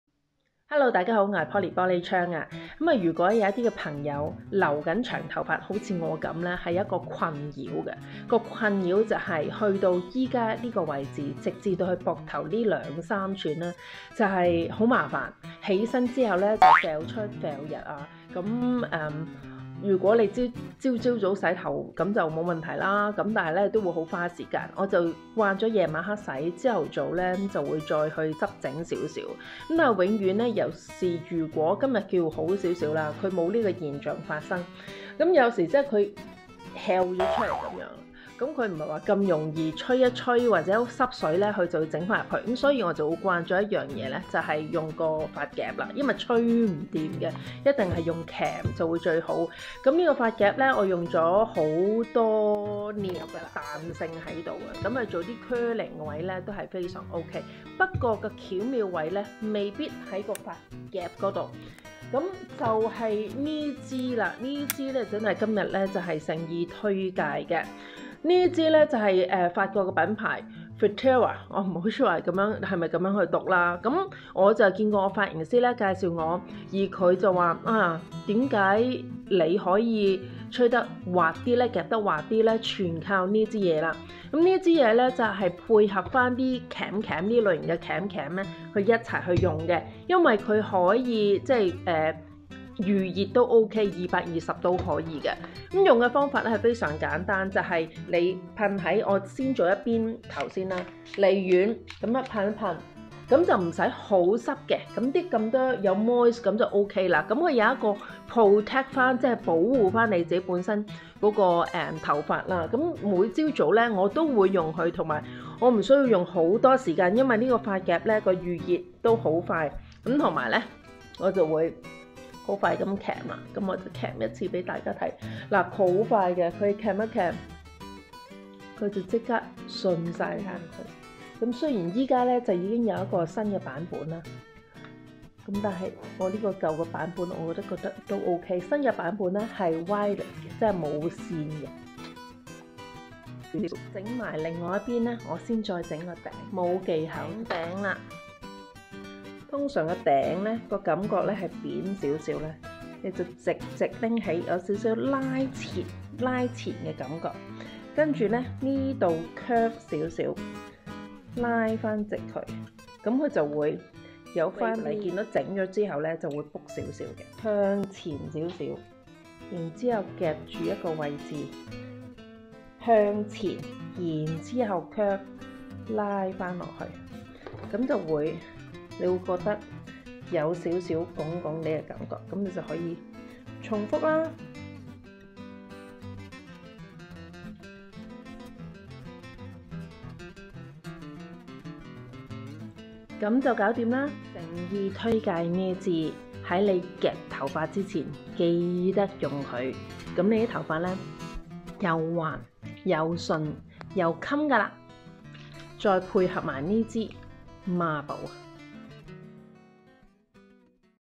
留長頭髮請注意#玻璃窗美容小帖士 polly玻璃窗￼￼順滑頭髮
如果你好似我咁留緊長頭髮係膊頭以上兩三吋最麻煩，早上起來￼經常頭髮蹺出蹺入，頭髮唔聽話。吹頭濕水都唔掂，咁就要用熨髮夾，我每朝早都用Amika熨髮夾，它有遠紅外線技術鎖緊頭髮水份
￼￼及鈦金屬造型器擁有100%的鈦金屬板及高效能的MCH陶瓷熱能技術，可保護髮質。
小貼士：是要配合法國品牌￼FURTERER /LISSEA￼￼ 絲滑順直護髮噴霧￼spray thermo - protecteur

燙髮之前￼及洗完頭吹頭髮之前可以用。效果顯著